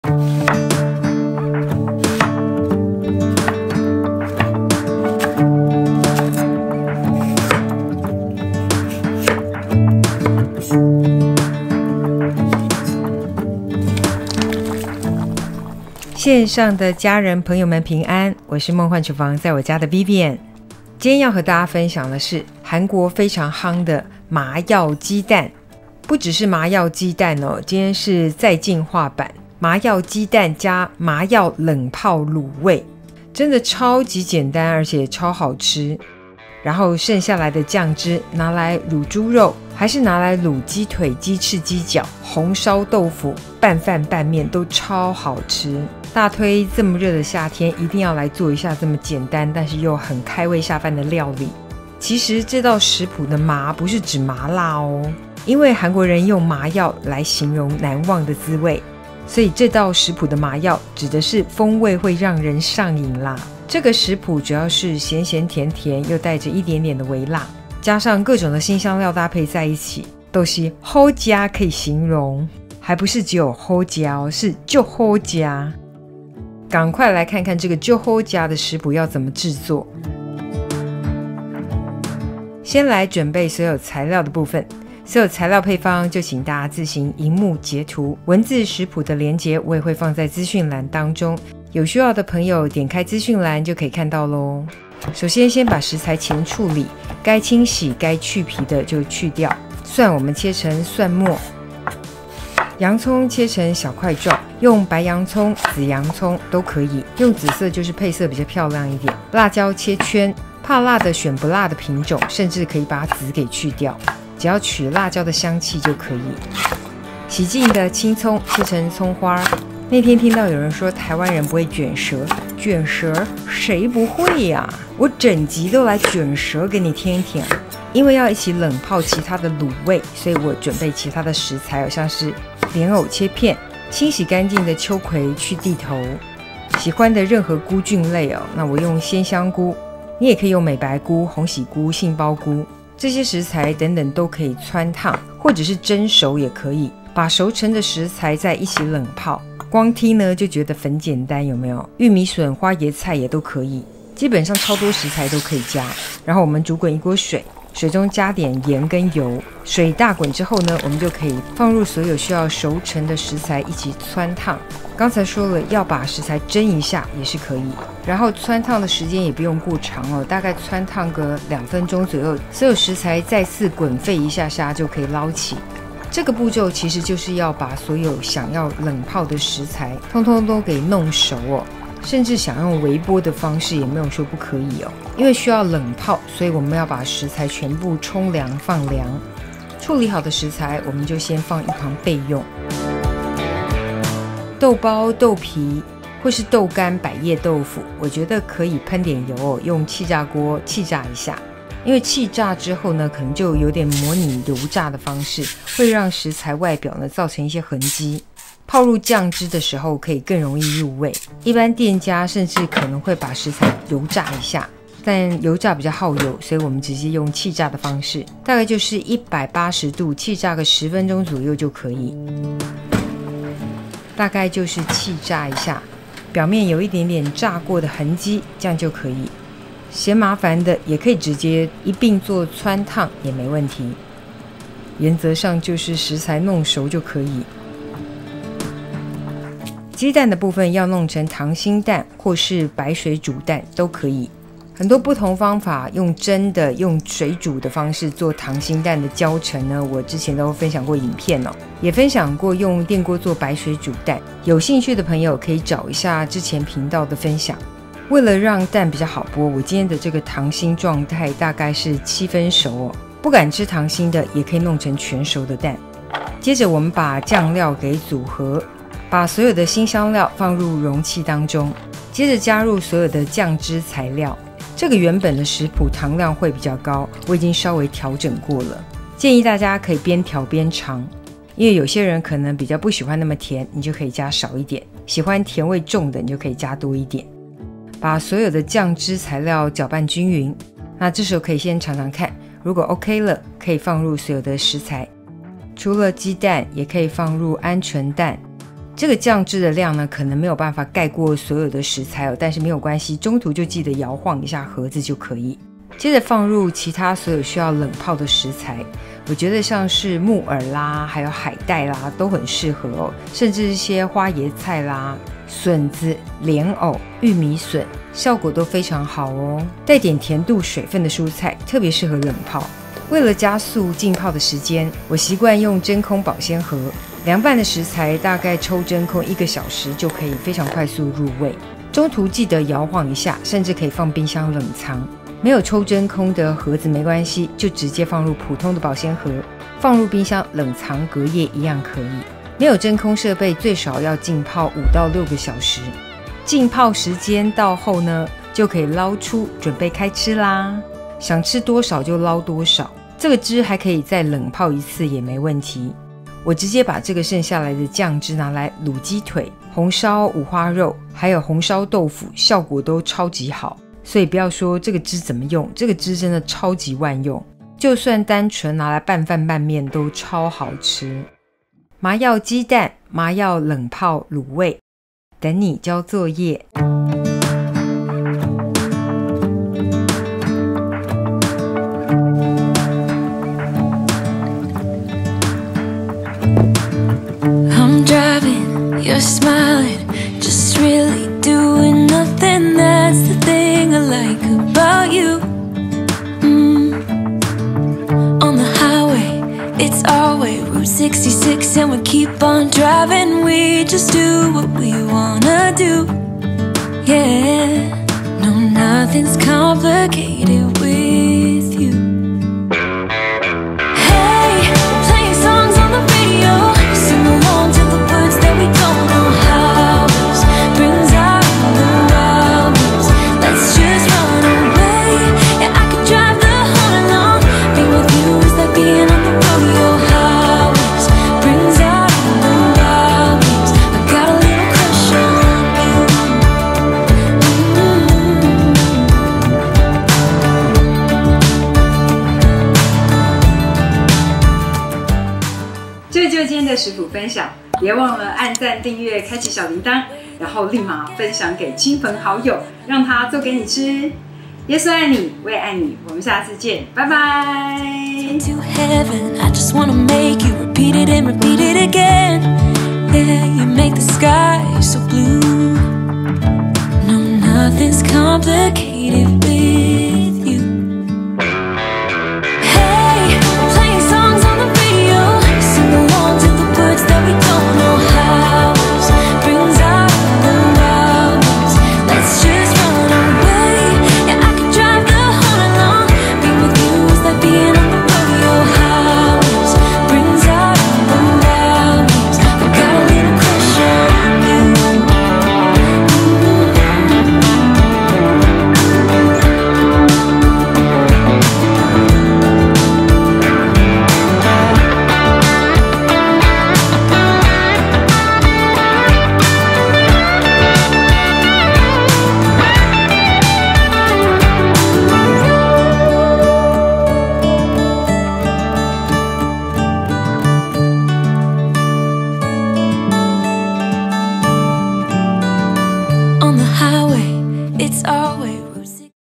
席捲韓國的麻藥雞蛋，再進化成麻藥冷泡滷味，真的是滷味的神仙吃法！Mayak Eggs & Braised Dishes(Korean Marinated Eggs）| 夢幻廚房在我家 ENG SUB
☆訂閱Vivian每週看新片，請記得開啟YouTube🔔通知！

🌸食譜：
蛋控、滷味控別錯過！風靡韓國的麻藥雞蛋，做法簡單，一吃就上癮。特別注意這道食譜的「麻」不是麻辣呦！而是韓國人用麻藥來形容難忘的滋味，就跟我們的「鴉片」粉圓有異曲同工之妙，總而言之就是會讓人上癮的涮嘴好吃啦！
麻藥雞蛋不只可以泡溏心蛋、水煮蛋、鵪鶉蛋也可以，剩下的醬汁不想浪費，直接用來浸泡其他食材做滷汁，做出冷泡滷味，風味真是讚！家常冷泡滷味冰箱有什麼就放什麼，大概鹽水雞的配料都可以放進去，又是一道精省的清冰箱料理啊！
或者直接拿來拌麵、泡飯甚至紅燒豆腐，通通都沒問題。這個滷汁濃縮了辛香料調味料的精華，同時味道並不濃烈而是略帶甘甜味，所以老少咸宜，是全家都輕鬆給過的口味！一鍋滷的麻藥冷泡滷味，吃得到最愛的麻藥雞蛋，也一起補充蔬菜纖維，滷汁還很萬用，歡迎分享你的麻藥滷汁變化菜色，等你交作業囉！

📌影片小撇步
1. 做溏心蛋雞蛋煮到時間要立刻丟冰水中冷卻降溫，否則會影響蛋黃溏心的程度。沖泡冷水至蛋殼縫隙中也會更好剝蛋殼。
2.一般浸泡至少5-6小時，隔天最好吃味道柔和濃郁，有尾韻蛋黃也會有奶油般的稠滑質地。但如果有真空設備的，抽真空可以加速入味時間，約真空1小時就能入味食用。
冰箱冷藏拿取時不沾到水，保存約5-7天不是問題。
3.想同時冷泡的食材只要想辦法熟成即可，可用氣炸、汆燙、煮熟或蒸熟即可冷泡食材，此為冷泡滷味。
4.糖的部份用楓糖、水麥芽、玉米糖漿、蜂蜜皆可，原本韓國當地食譜的糖醬油清水份量為1:1:1，但我覺得有點太甜膩，修改成較少的範圍，大家可依據喜歡的甜度微調。如果想醃漬的食材更多，可按比例微調增加醬油、清水、楓糖的量。真的想再減低糖量也可以沒問題。自己嚐嚐喜歡最重要！

👩🏻‍🍳 食材 Recipes
雞蛋10顆
鵪鶉蛋10顆
🌸想一起冷泡其他食材也可以菇類、木耳、蓮藕、玉米筍、秋葵、豆乾、豆皮、海帶川燙或蒸熟或氣炸鍋170度C炸一下至外表有不平整的狀態放涼即可一起冷泡
《醬汁材料》
醬油250克
飲用水250克
楓糖150-185克(或水麥芽或蜂蜜150-185克)
蒜頭4顆
青蔥2根
熟白芝麻1-2大匙
辣椒2條(依自己喜歡的辣度)
紫洋蔥1/4顆
香菜1-2株
冷壓麻油3大匙
🌸 糖的部份用楓糖、水麥芽、玉米糖漿、蜂蜜皆可，原本韓國當地食譜的糖醬油清水份量為1:1:1，但我覺得有點太甜膩，修改成較少的範圍，大家可依據喜歡的甜度微調。如果想醃漬的食材更多，可按比例微調增加醬油、清水、楓糖的量。

Eggs(10)
quail eggs(10)
🌸 Other ingredients for cold-braising: Mushrooms, jelly ears, lotus roots, baby corns, okras, dried tofu, fried tofu skin, sea tangle, etc. Blanch or steam them. Air-fry them a little with 170°C with the surface a little uneven. Must cool down all the ingredients, and you can cold-braise them.
《Sauce》
Soy sauce(250g)
drinking water(250g)
maple syrup(150-185g)(you can replace maple syrup with maltose syrup or honey(150-185g))
garlic(4 cloves)
scallions(2)
white sesame(1-2tbsps)
chili(2)(add accordingly)
red onion(1/4)
cilantro(1-2strains)
cold-pressed sesame oil(3 tbsps)
🌸You can replace sugar with maple syrup, corn syrup, maltose syrup or honey. The original proportion in the Korean recipe for sugar, soy sauce and water is 1:1:1, but I find it woo sweet. I adjusted a little, you can also adjust it, too. If you want to marinate more ingredients, you can add the amount.

☘️料理名稱＆做法不一定正統，食譜純粹以個人經驗改良分享 ，請多包涵指教。

🌹About Dream Chef Home 夢幻廚房在我家🌹